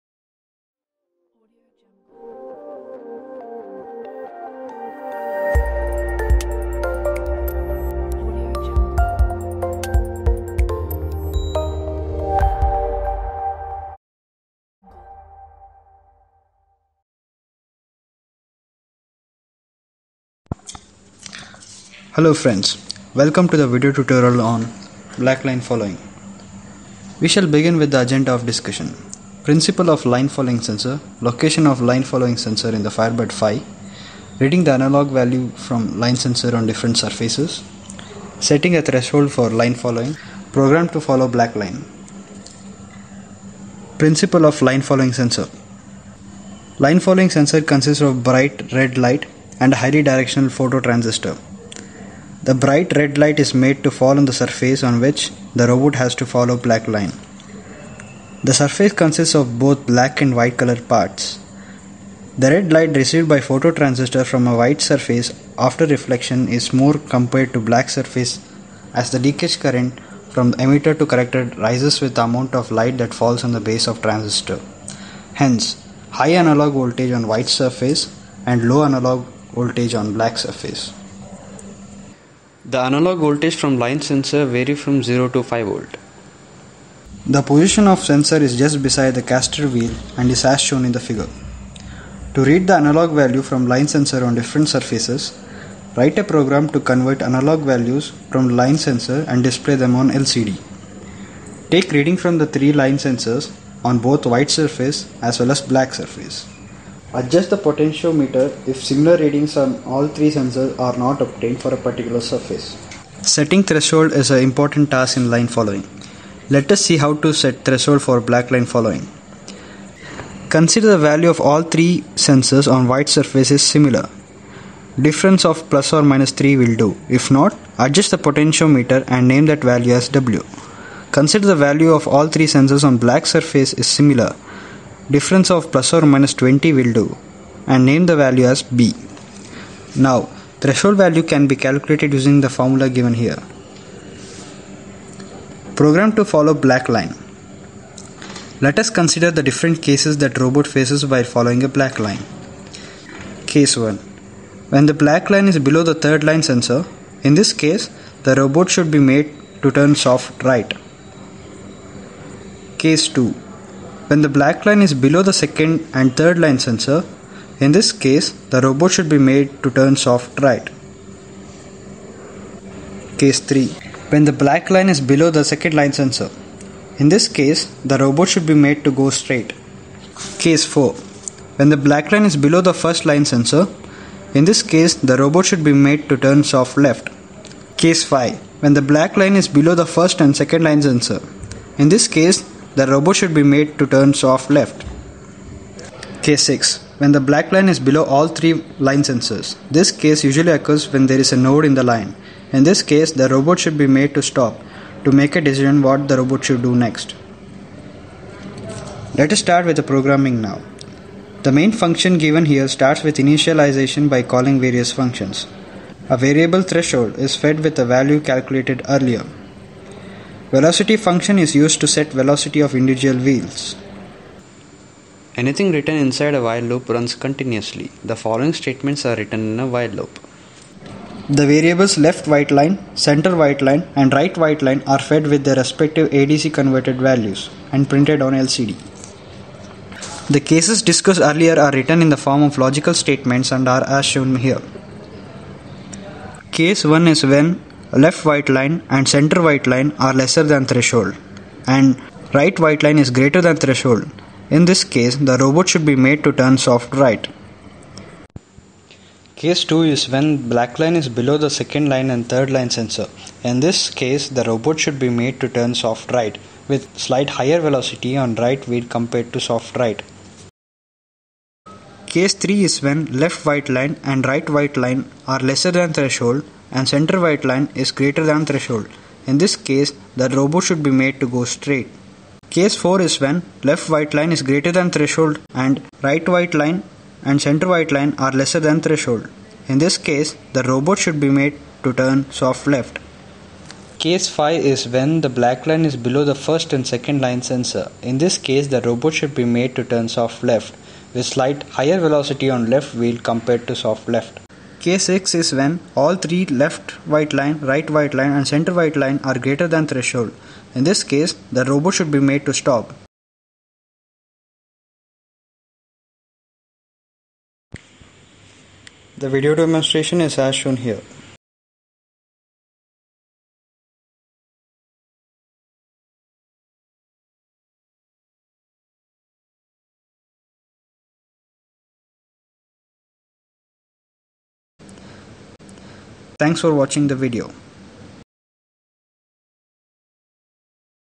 eLSI: Blackline following- Tutorial
A brief tutorial on black line following using Firebird V.